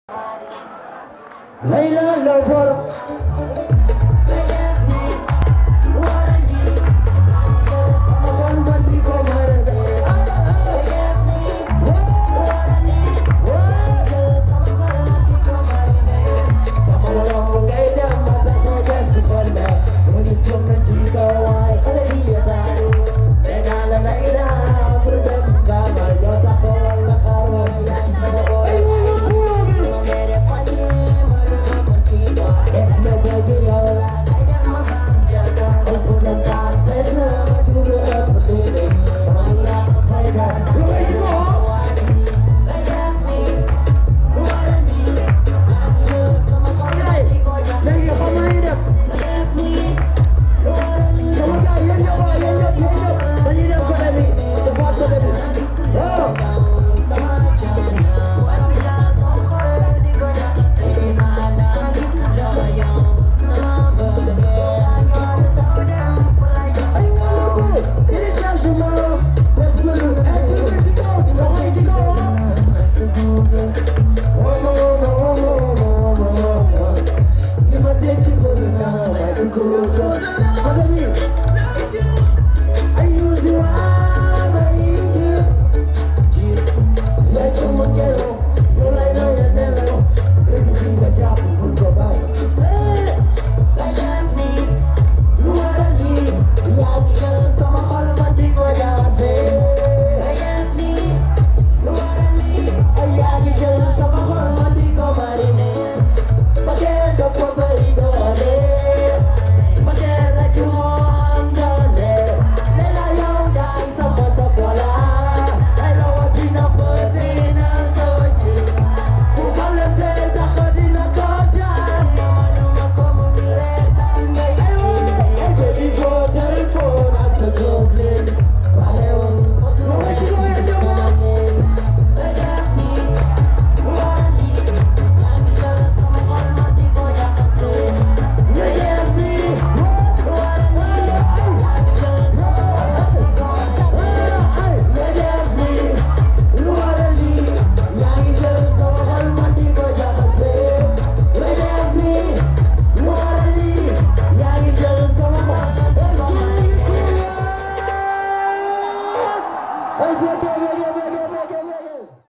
LE SHOW SOMPTUEUX DE AMEDY MOUSTAPHA A MBOUR
Amedy Moustapha l'artiste a la voix d'or en Play-Back au stade Caroline faye de Mbour